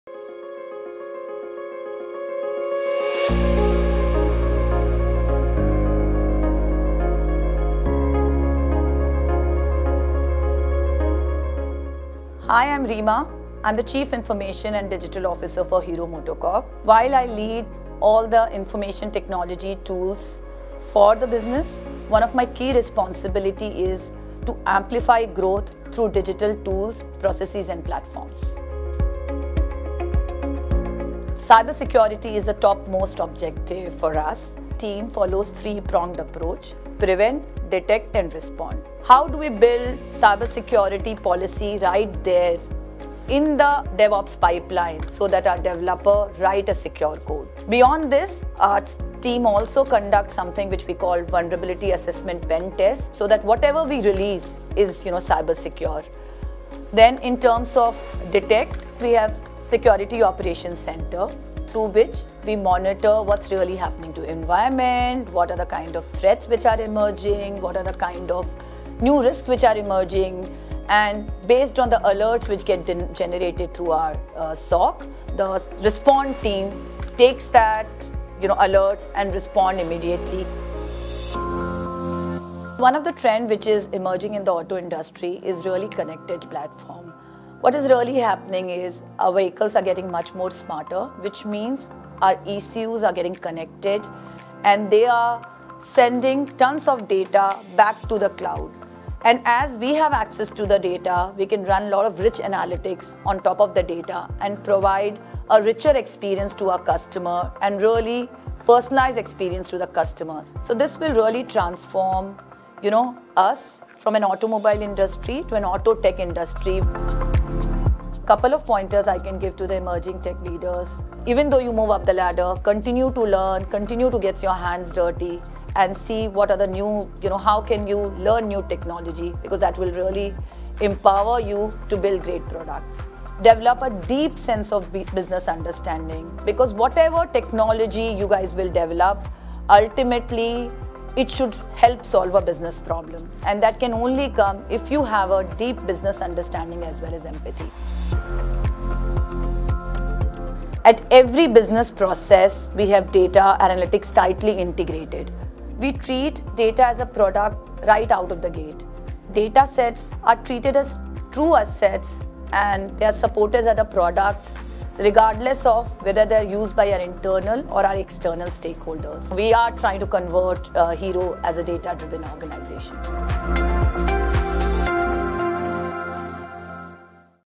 Forbes India presents 'The Data Circle' in association with Snowflake | Reema Jain Hero MotoCorp
With more than two decades of experience working in the IT industry, Reema Jain is a technology expert delivering business growth and transformation through Cloud, AI, Analytics and DevOps.

As the Chief Information and Digital Officer at Hero MotoCorp, Jain leads the information technology tools for the organisation, amplifying growth through digital tools, processes and platforms. With emerging trends in the Automobile industry, Data analytics and Cyber security are paramount to convert it into the Auto Tech industry.

Jain believes that continuous learning and developing a deep sense of business understanding is the key to solving business problems.